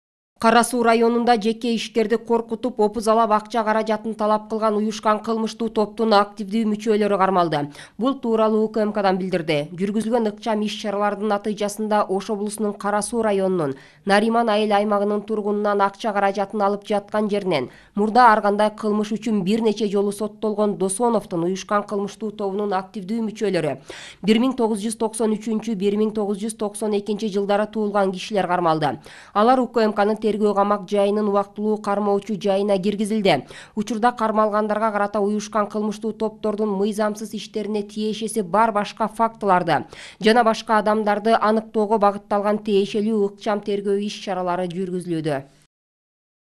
Кылмыштуу топтун мүчөлөрү кармалды
Кара-Суу районунда жеке ишкерлерди коркутуп, опузалап акча каражатын талап кылган уюшкан кылмыштуу топтун активдүү мүчөлөрү кармалды. Бул тууралуу УКМК билдирди. Жүргүзүлгөн ыкчам иш-чаралардын натыйжасында, Ош облусунун Кара-Суу районунун Нариман айыл аймагынын тургунунан акча каражатын алып жаткан жеринен мурда ар кандай кылмыштар үчүн бир нече жолу соттолгон, Досоновдун уюшкан кылмыштуу тобунун активдүү мүчөлөрү, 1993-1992-жылдары туулган кишилер кармалышты. Алар УКМКнын тергөө камак жыйынын убактылуу кармоочу жайына киргизилди. Учурда кармалгандарга карата уюшкан кылмыштуу топтордун мыйзамсыз иштерине тиешеси бар башка фактыларды жана башка адамдарды аныктоого багытталган тиешелүү ыкчам тергөө иш-чаралары жүргүзүлүүдө.